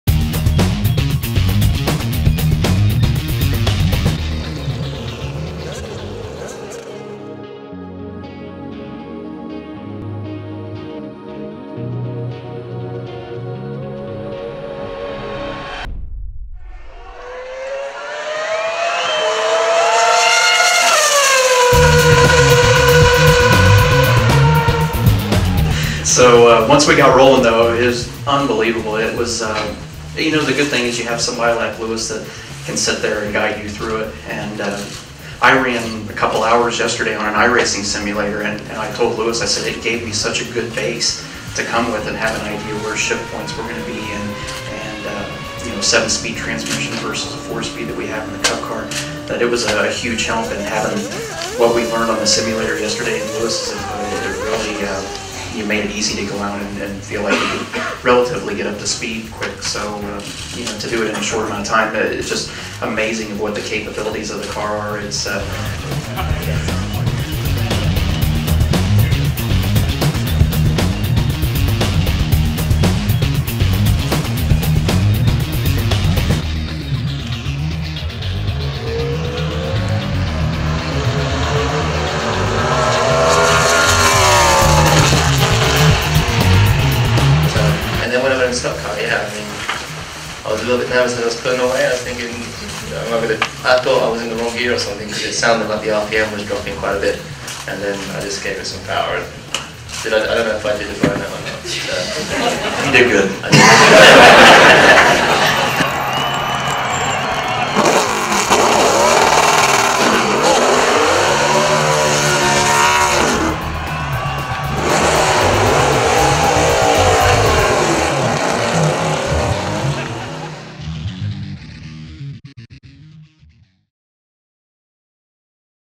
Tony Stewart & Lewis Hamilton Car Swap Trailer.wmv
Tony Stewart uses the iRacing software to prepare to drive Lewis Hamilton's 2008 Championship winning Formula 1 racecar.  Both drivers swapped racecars at the historic Watkins Glen International race circuit in June.  Tony discusses how iRacing helped him prepare for this once in a lifetime opportunity.